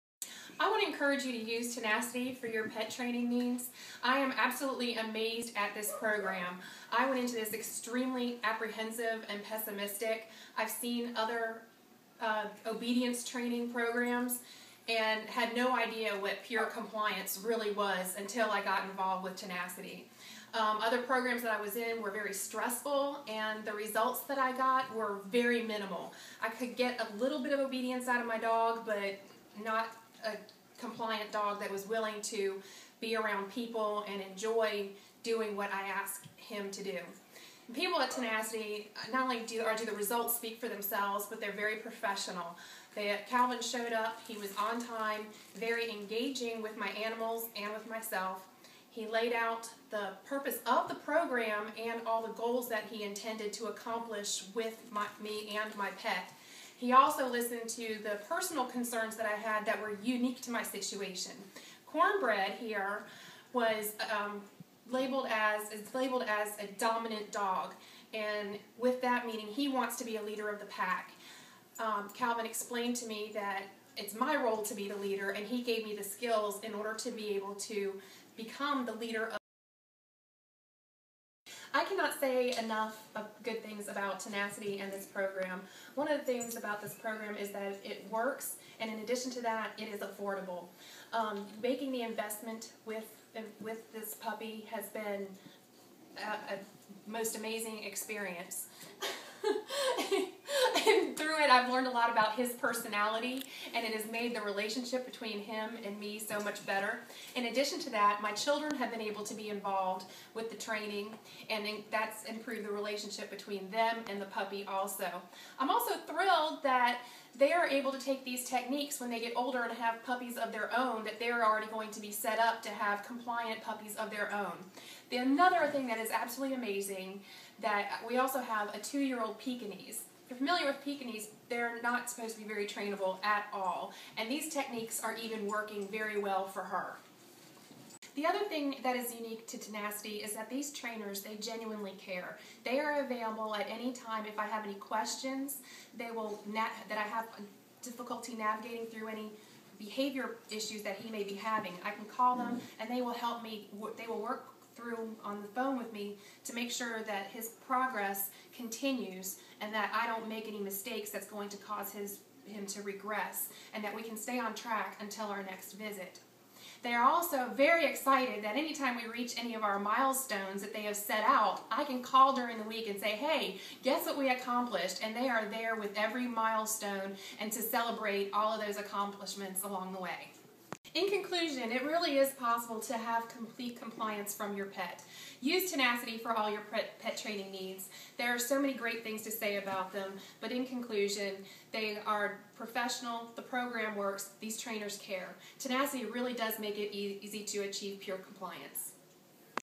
Bloodhound training testimonial
This is one of our wonderful clients! We worked with Dawn and her family to help with their pack structure. This is what our clients have to say about us. If you would like to have your dog trained by Tenasity give us a call at Dawn and her entire family have been an absolute pleasure to work with!